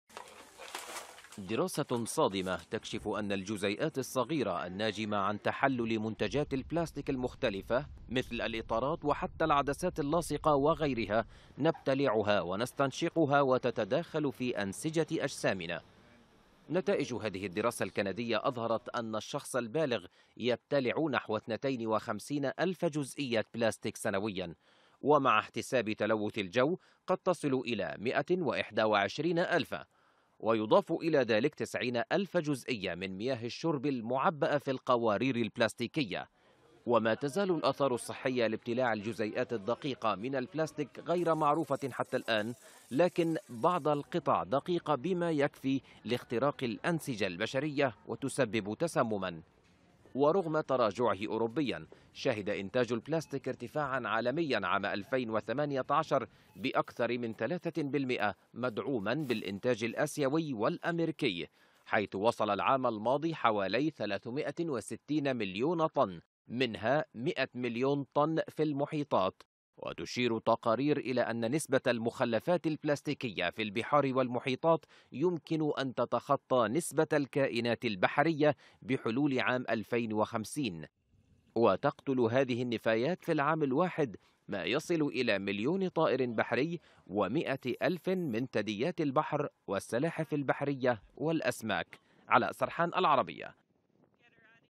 تقرير: البشر يبتلعون ويستنشقون عشرات آلاف جزئيات البلاستيك س
أظهرت أبحاث جديدة أن البشر يبتلعون ويستنشقون عشرات آلاف جزئيات البلاستيك سنويا.

ـــــــــــــــــــــــــــــــــــــــــــــــــــــــــــــــــــــــــــــــــــــــــــــــــــــــــــــــــــــــــــــــــــــ